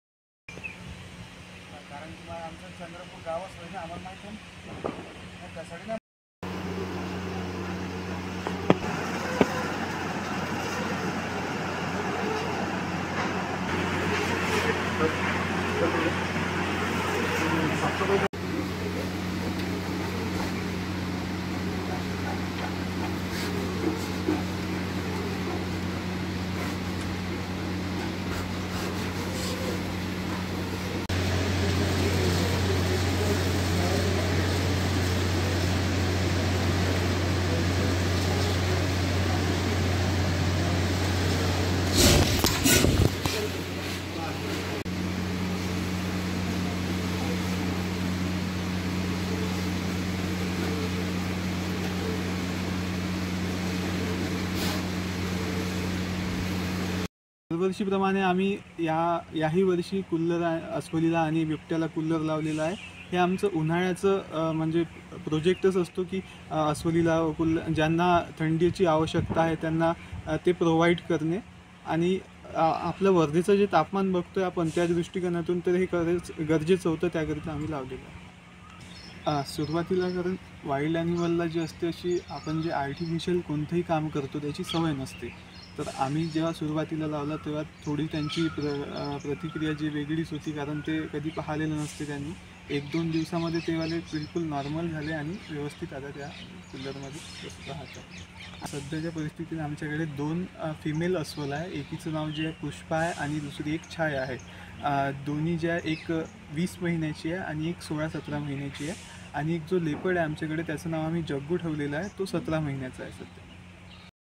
WARDHA | आता जग्गू पाठोपाठ पुष्पा आणि छायाला थंड कुलरची हवा
वर्ध्यामध्ये दिवसेंदिवस तापमान वाढत आहे. या तापमानाचा फटका मानवासह प्राण्यांनाही बसत आहे. जिल्ह्यात 40 ते 44 अंश सेल्सिअस पर्यंत तापमान पोहोचले आहे. वर्धा येथील जग्गू बिबट्यासह आता छाया आणि पुष्पा या दोन्ही अस्वलांना कुलरची हवा दिली जात आहे. उन्हाच्या झळांपासून सुटका होण्यासाठी कुलरची व्यवस्था करण्यात आली असल्याचे ऋषिकेश गोडसे यांनी सांगितले आहे. जेव्हा पहिल्यांदा हे कुलर लावण्यात आले होते, तेव्हा त्यांची प्रतिक्रिया ही वेगळीच होती. परंतु एक-दोन दिवसांनी त्यांना या कुलरची सवय झाली असल्याचे सुद्धा प्राणी मित्रांनी सांगितले आहे.

BYTE  - प्राणीमित्र ऋषिकेश गोडसे